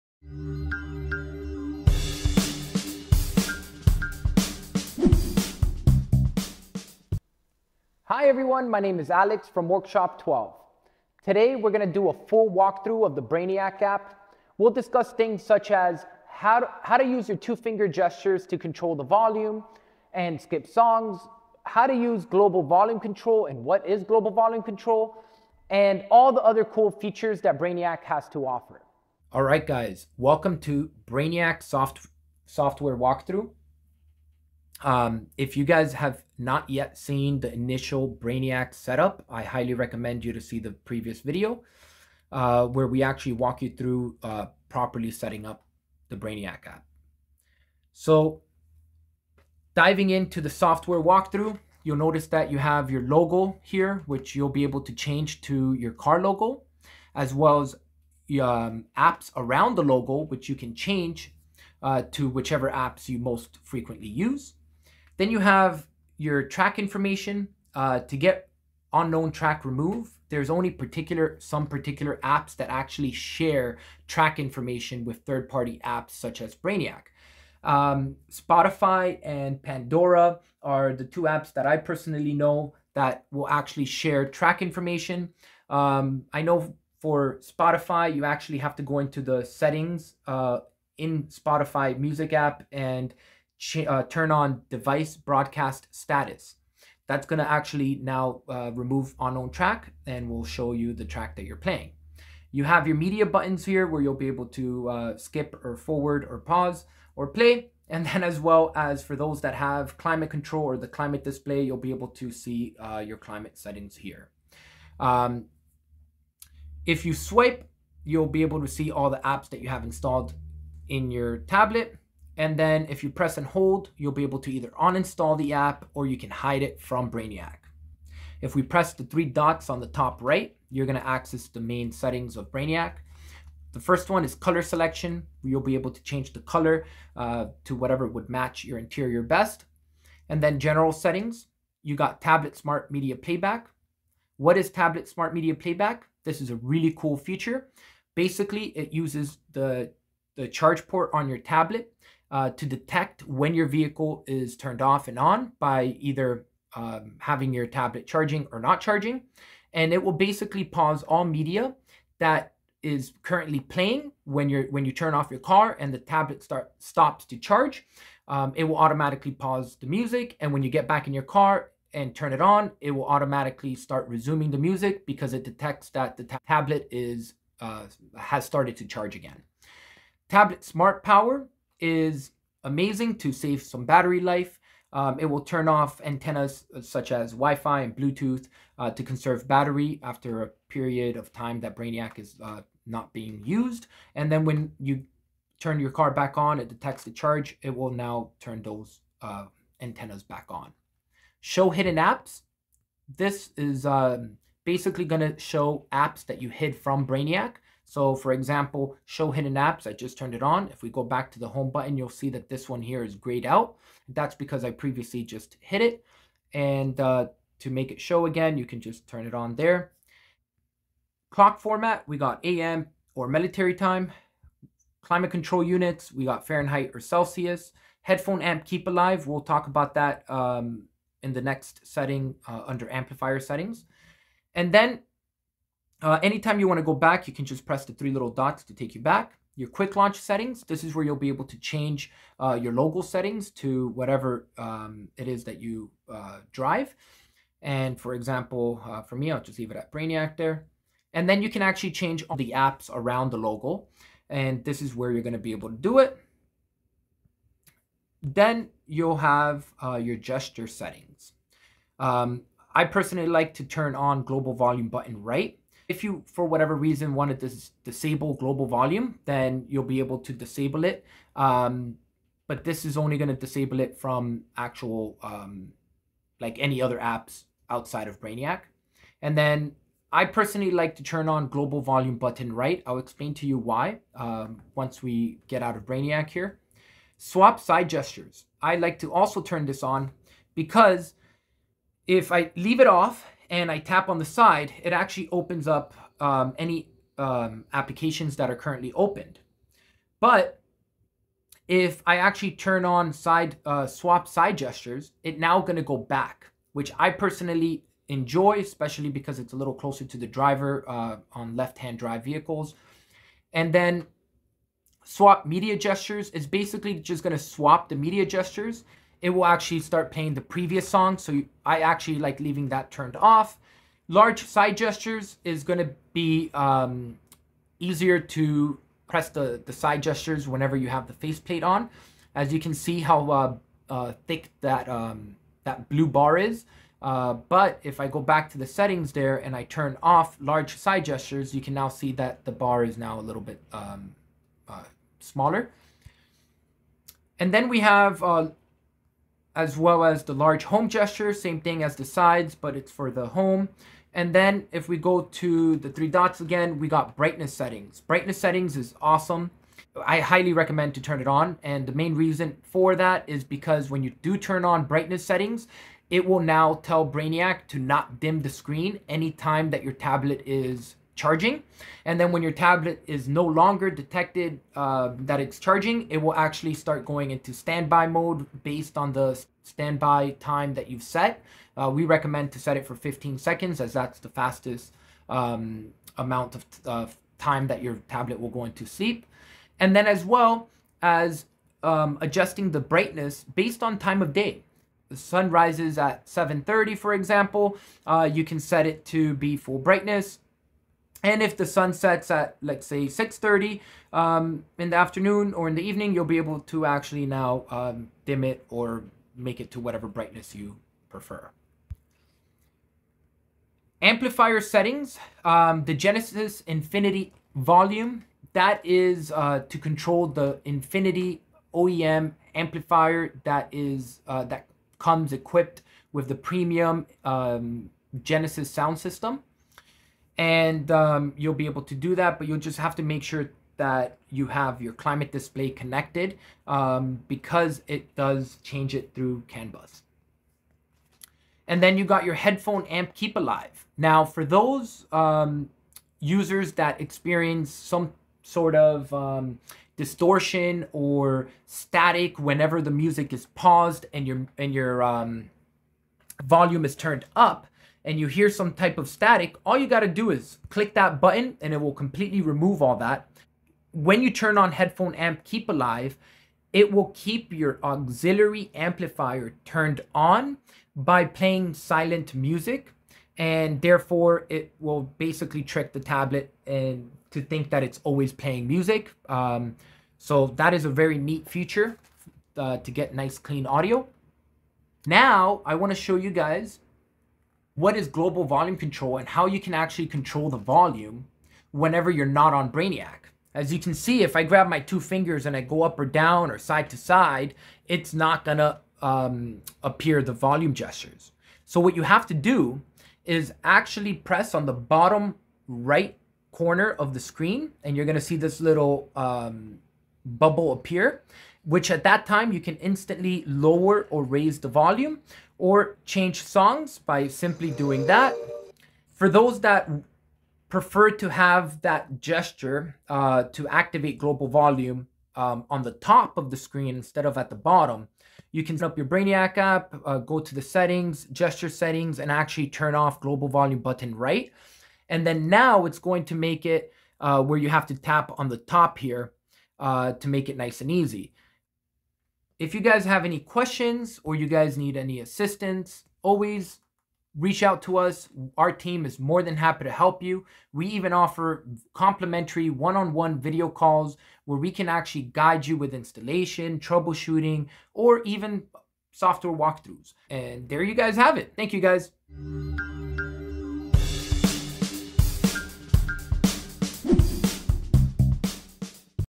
Brainiac Walkthrough v3.9
Take a tour of the Brainiac home screen launcher for Android designed specifically for Android tablets used with Workshop 12 Brainiac plastics.